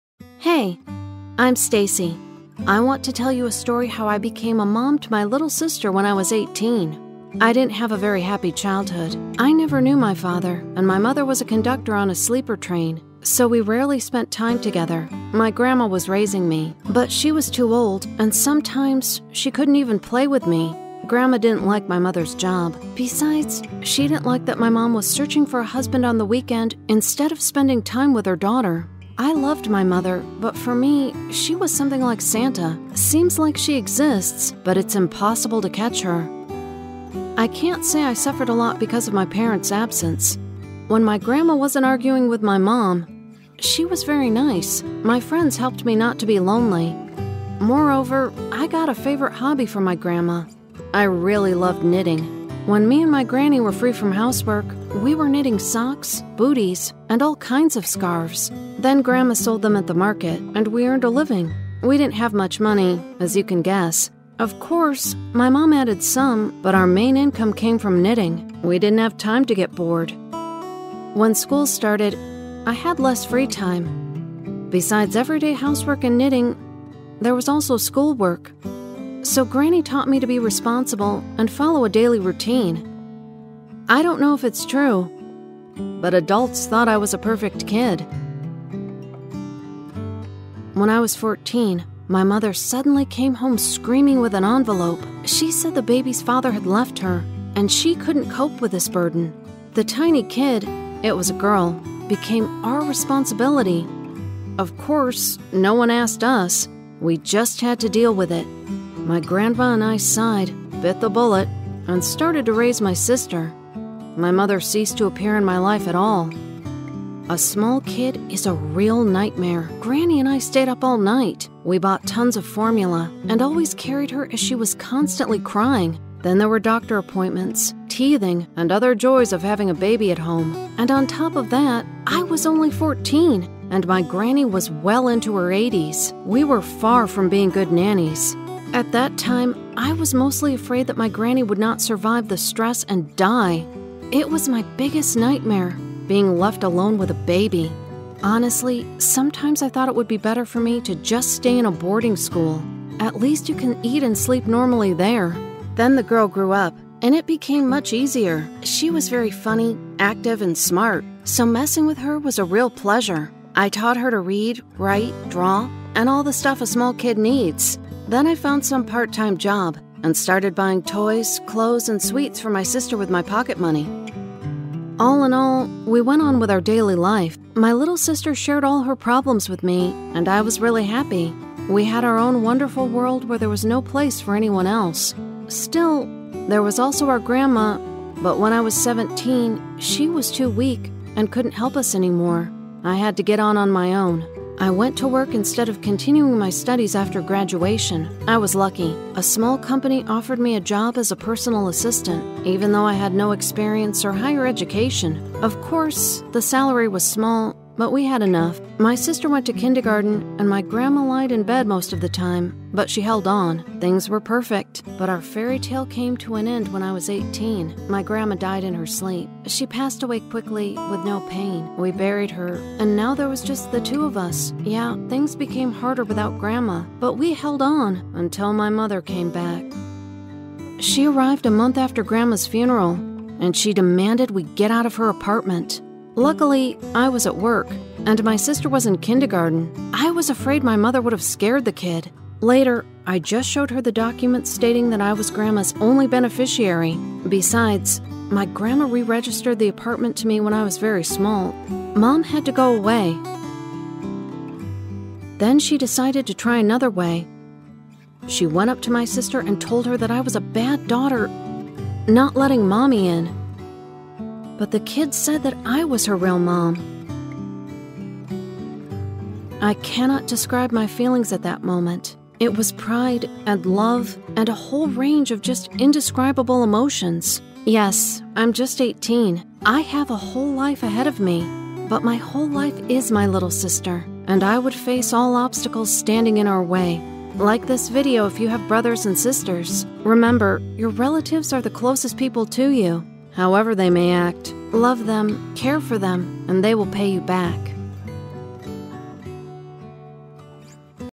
My mother left me with a baby (my sister) in my arms
Hey! Today Stacy want to tell you a story how she became a mum for my little sister when she was 18.

I didn’t have a very happy childhood. I never knew my father and my mother was a conductor on a sleeper train. So we rarely spent time together. My grandma was bringing me up. But she was too old and sometimes she couldn’t even play with me. Grandma didn’t like my mother's job. Besides, she didn’t like that my mum was searching for a husband at the weekend instead of spending her time with the daughter. I loved my mother, but for me she was something like Santa – seems like she exists but it’s impossible to catch her.

I cannot say I suffered a lot because of parents’ absence. When my grandma wasn’t arguing with my mum, she was very nice. My friends helped me not to feel lonely. Moreover, I got a favourite hobby from my grandma – I really loved knitting.

When me and granny were free from the housework, we were knitting socks, booties and all kinds of scarves. Then grandma sold it in the market and we earned a living. There were not much money in my family, as you can guess. Of course, my mum added something, but the main income came from the knitting. We didn’t have time to get bored and suffer.

When the school had started, I had less free time. Beside everyday housework and knitting there was also school homework. So, granny taught me to be responsible and follow the daily routine. I don't know if it was right but adults thought I was a perfect kid.

When I was 14, my mother suddenly came back home with a screaming envelope. She said the baby's father had left her, and she couldn't cope with this burden. The tiny kid (it was a girl) became our care and responsibility. Of course, no one asked us – we were just pinned down to facts. My grandma and I sighed, bit the bullet and started to raise my sister. My mother ceased to appear in my life at all.

A small kid is a real nightmare. Granny and I stayed up all night. We bought tons of kids’ mixes and al-ways carry the baby on our arms as she was constantly crying. Then there were lines in the hospital, teething and other «pleasures» of having a baby at home. And on top of that - I was only 14, and my granny was well over her 80s. We were far from being good nannies.

At that time I was mostly afraid that my granny would not survive the stress and die. It was my biggest nightmare – being left alone with a baby. Honestly, sometimes I thought it would be better for me just to stay in a boarding school. At least you can normally eat and sleep there.

Then the girl grew up and it became much easier. She was very funny, active and smart - so messing with her was a real pleasure. I taught her to read, write, draw and all the stuff a small kid needs. Then I found some part-time job and I started buying toys, clothes and sweets for my sister on my pocket money.

All in all, we went on our daily life. My little sister shared all her problems with me, and I was really happy. We had our own wonderful world where there was no place for anyone else. Still, there was also our grandma, but when I was 17 she was too weak and could not help us anymore. I had to get on my own.

I went working instead of continuing my studies after school. I was lucky - a small company offered me a job as a personal assistant, even though I had no experience and higher education. They didn’t make me any indecent hints. Of course, the salary was small but we had enough. My sister went to a kinder-garten and my grandma lied in bed most of the time, but she held on. Things became perfect.

But our fairytale came to an end when I was 18. My grandma died in sleepping. She passed away quickly with no pain. We buried her and now there were just the two of us. Yeah, things became harder without grandma, but we held on. Until my mother came back.

She arrived a month later after grandma's funeral. And she demanded we get out of her apartment. Luckily, it was my working day and my sister was in the kindergarten. I was afraid my mother would have scared the kid. Later I just showed her the document stating that I was the only grandma's succes-sor. Besides, my grandma re-registered the apartment to me when I was very small. Mum had to go away.

Then she decided to do the other way. She went up to my sister and told her that I was a bad daughter not letting the mummy in. But the kid said that I was her real mum.

I cannot describe my feelings at that moment. It was pride, and love, and a whole range of just inde-scribable emotions. Yes, I'm just 18, I have a whole life ahead, but my whole life is my little sister. And I would face all the obstacles standing in our way.